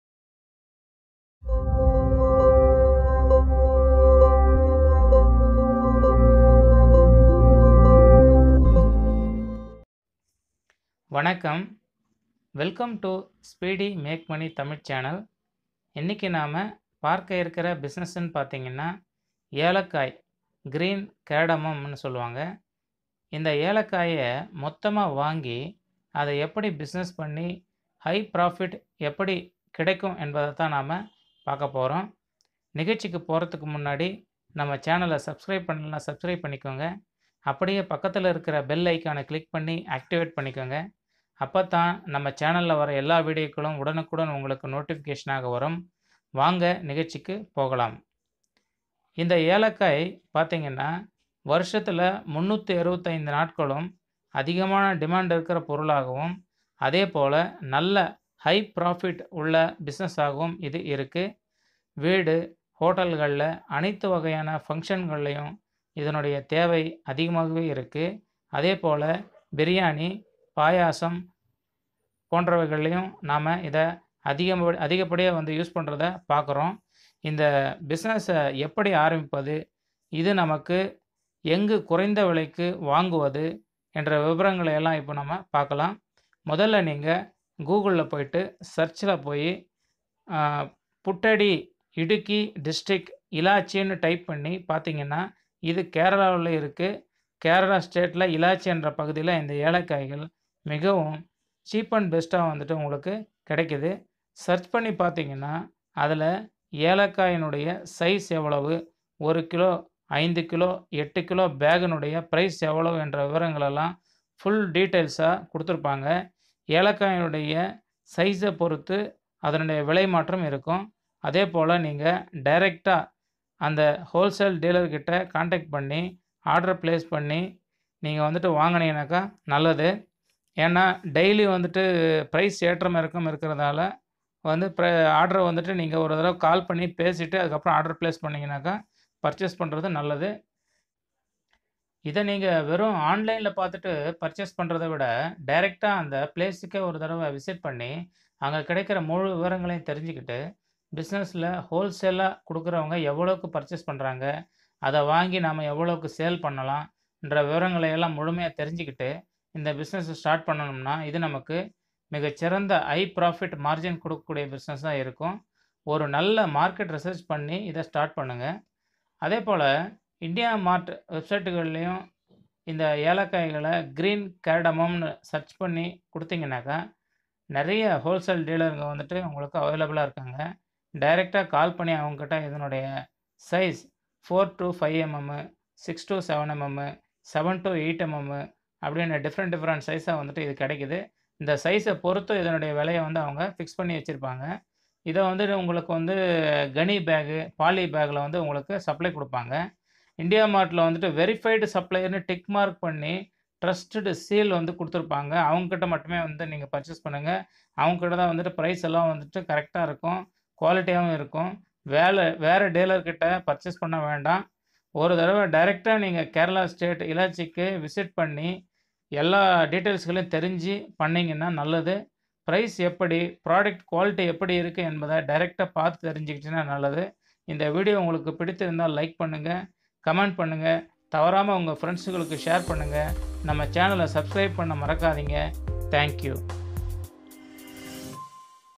Business ideas / small business ideas in tamil / Speedy make money tamil
Business ideas / small business ideas in tami

In this video I had covered a profitable idea in India which you can start from home and Start as small scale industry in anywhere india, This new business idea is a low investment and high profitable business, This business recognize as small scale business idea in India,